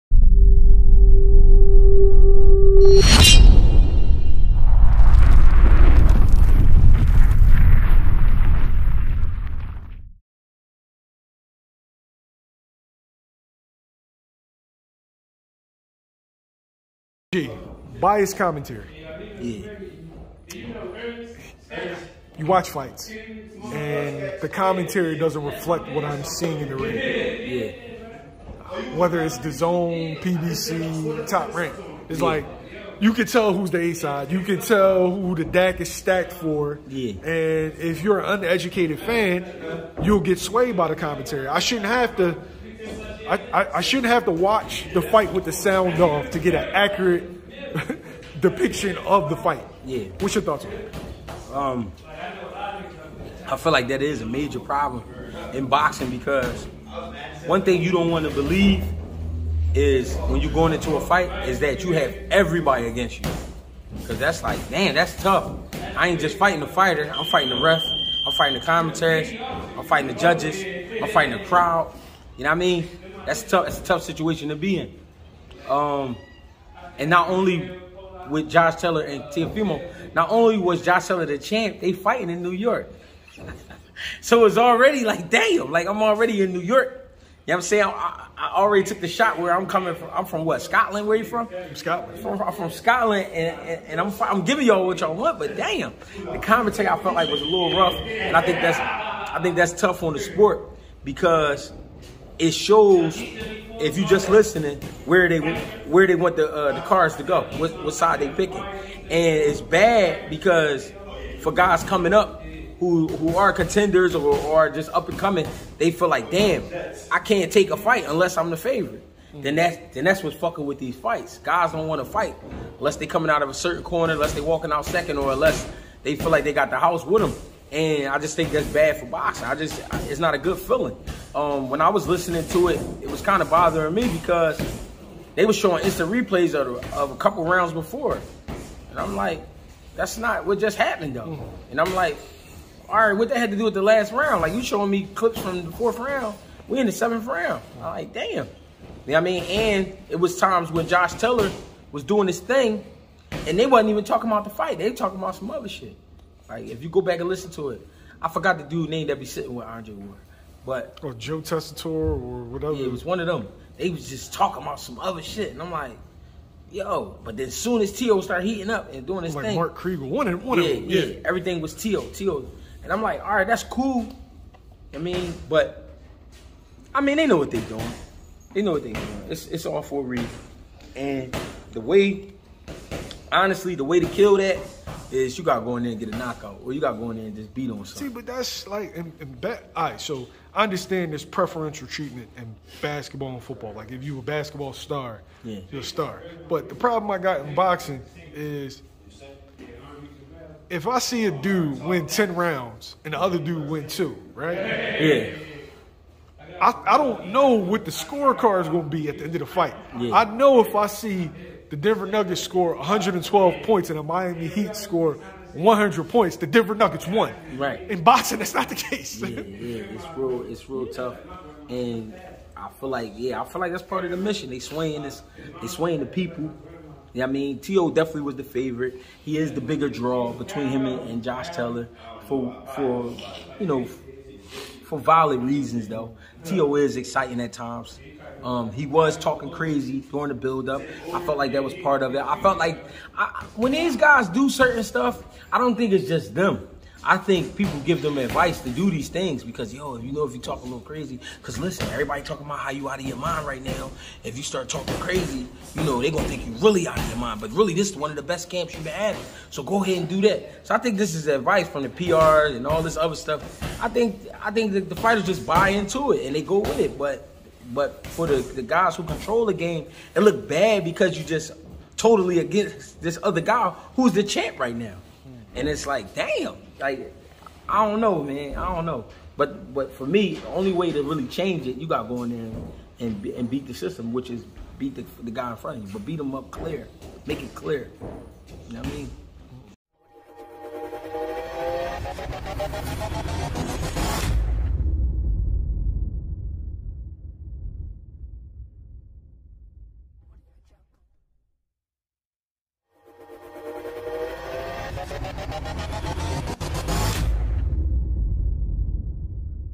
"IT'S A MAJOR PROBLEM IN BOXING" Greg Hackett GOES IN on Biased Commentators
Trainer/Fighter Greg Hackett shares his thoughts on Biased Commentary in the sport of Boxing across multiple platforms.